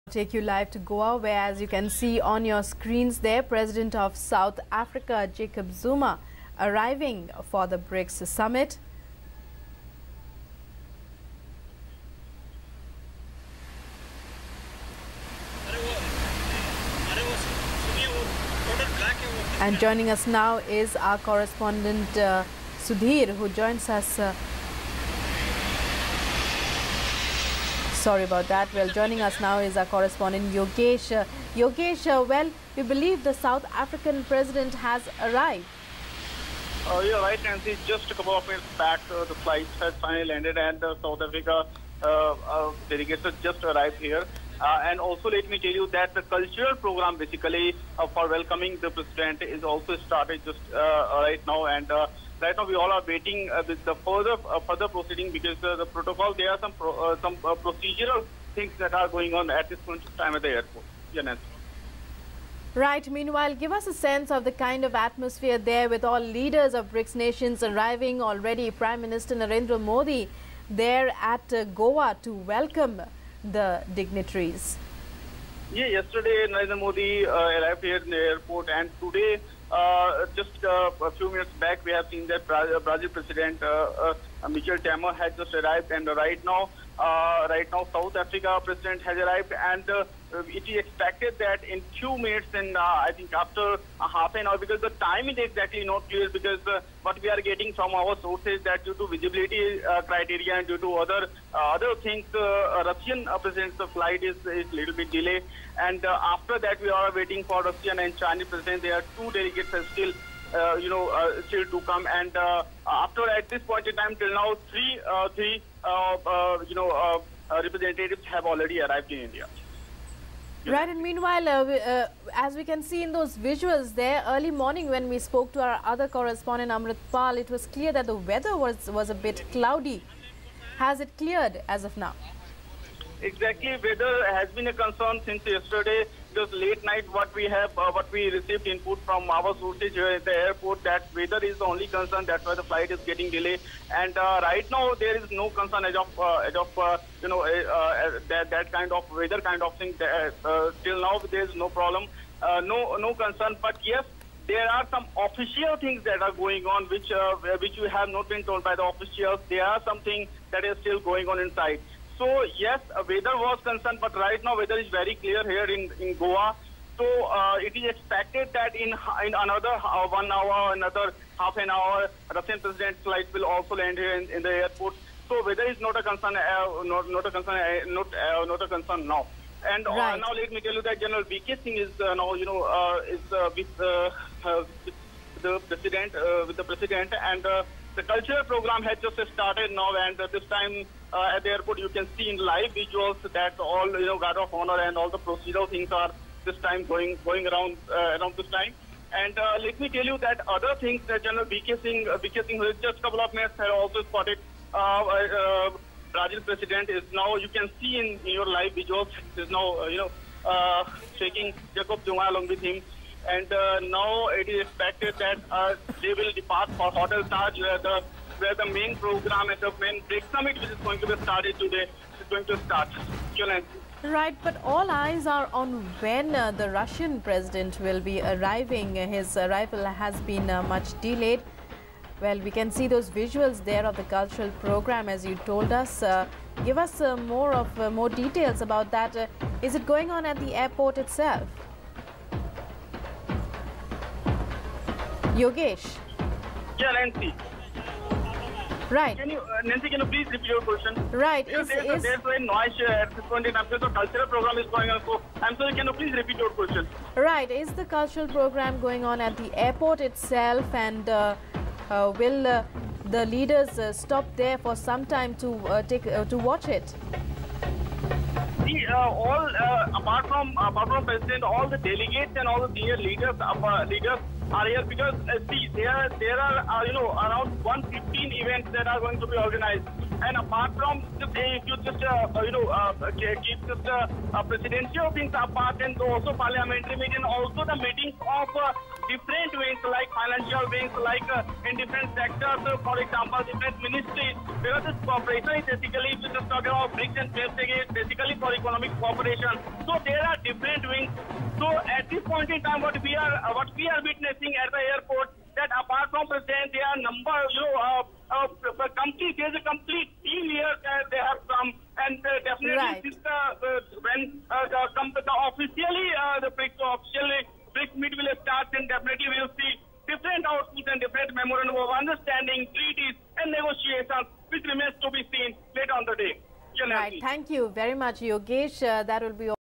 South African President Jacob Zuma arrives at Goa Airport
ब्रिक्स सम्मेलन में शिरकत करने के लिए दक्षिणी अफ्रिका के राष्ट्रपति जैकब जुमा
 गोवा पहुंचे